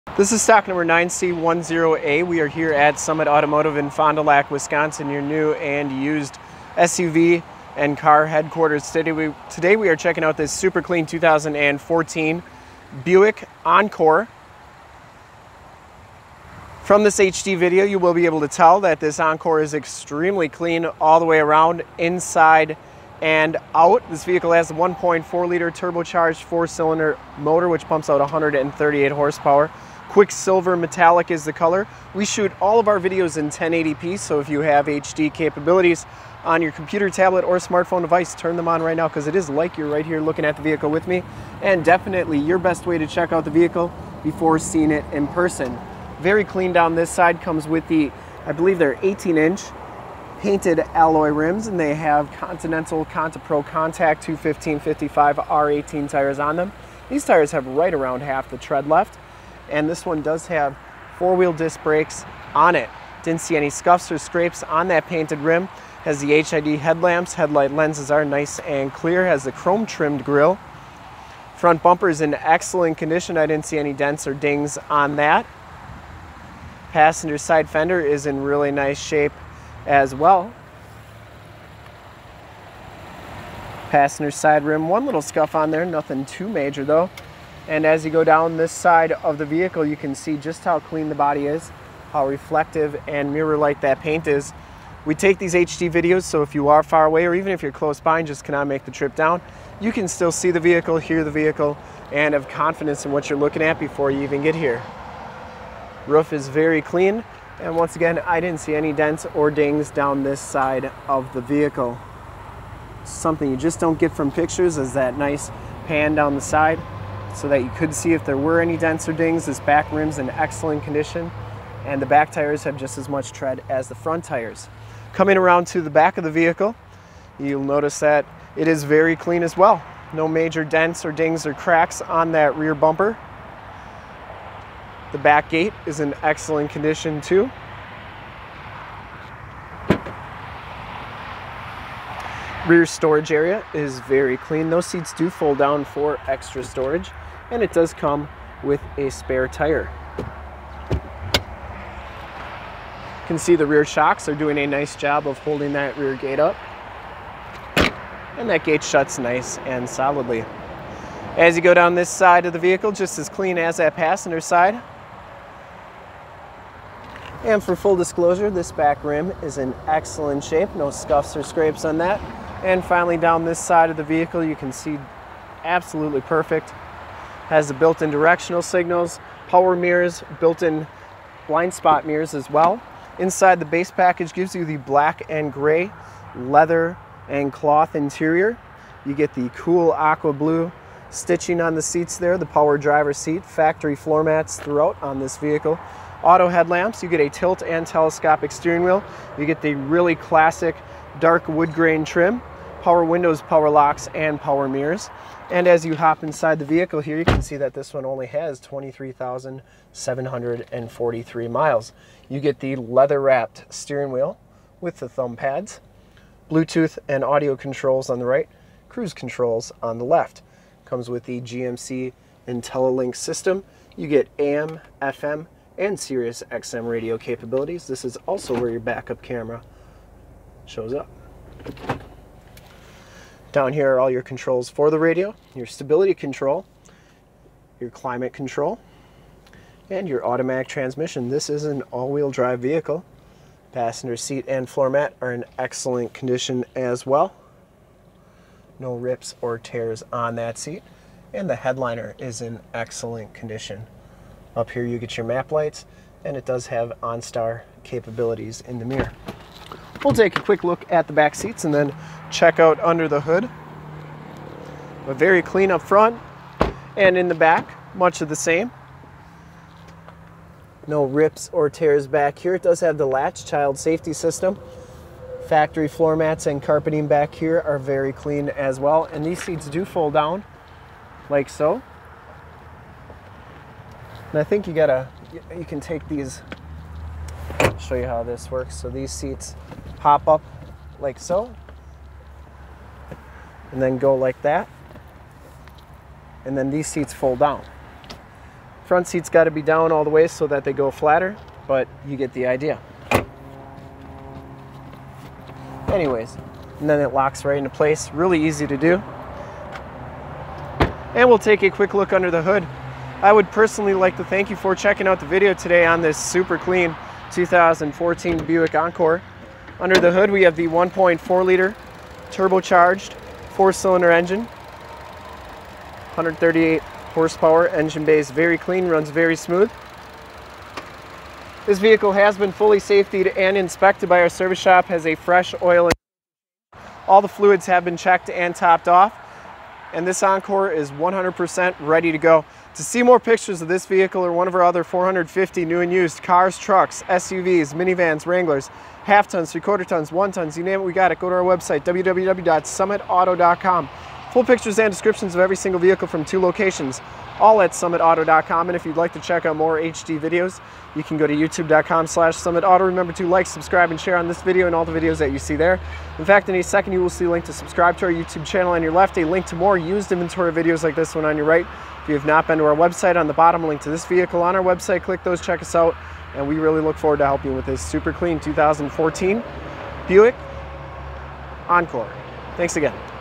2014 BUICK ENCORE AWD FOND DU LAC WISCONSIN QUICKSILVER METALLIC SOLD! 9C10A
2014 BUICK ENCORE AWD IN QUICKSILVER METALLIC

2014 BUICK ENCORE AWD IN QUICKSILVER METALLIC

STOCK: 9C10A
PRICE: $14,999
MILES: 23,732
MAKE: BUICK
MODEL: ENCORE
VIN: KL4CJESB3EB595616
LOCATION: FOND DU LAC OSHKOSH WISCONSIN, 54937 TRUCKS ON 41

CLEAN TITLE HISTORY! 1.4 Liter DOHC I4 Turbocharged Engine, 138 Horsepower, Base Package, 6 Speed Automatic Transmission, Automatic Transmission Center Console Shifter, All Wheel Drive Four Wheel Drive AWD, Reverse Backup Camera Rearview Camera, Onstar System, Power Driver's Seat, Gray Cloth Seats, Bucket Seats, 2nd Row Bench Seating, Power Mirrors With Built-in Blindspot Mirrors, Power Mirrors with Built-in Directional Signals, Stabilitrak Traction Control, Continental Contiprocontact P215/55 R18 Tires, Painted Alloy Rims Premium Wheels, Front Wheel Disc Brakes, HID High Intensity Discharge Headlights, Buick IntelliLink Touchscreen Radio, AM / FM Radio Tuner, Sirius/XM Satellite Radio Capabilities Sirius / XM, CD Player, Bluetooth, Hands-Free Phone Controls Blue Tooth, Auxiliary MP3 Jack Portable Audio Connection, USB Jack Portable Audio Connection, Keyless Entry System, Manual Raise Rear Gate, Rear Window Defroster, Adjustable Height Seatbelts, Driver and Passenger Front Air Bags, L.A.T.C.H. Child Safety System, Side Curtain Air Bags SRS Safety Restraint System, 2nd Row Power Windows, Multi-Function Steering Wheel Controls, Compass, Outside Temperature Display and Mileage Display, Factory Floormats, Woodgrain Dash And Door Trim, Air Conditioning AC, Cruise Control, Power Locks, Power Windows, Automatic Headlights Autolamp, Tilt/Telescope Steering Wheel, QuicksSilver Metallic! CLEAN AUTOCHECK! Very very clean inside and out! This is one of the sharpest 2014 Buick Encore suvs we have ever had on our lot! Make your move before this super clean ride is gone!

STOCK: 9C10A
PRICE: $14,999
MILES: 23,732
MAKE: BUICK
MODEL: ENCORE
VIN: KL4CJESB3EB595616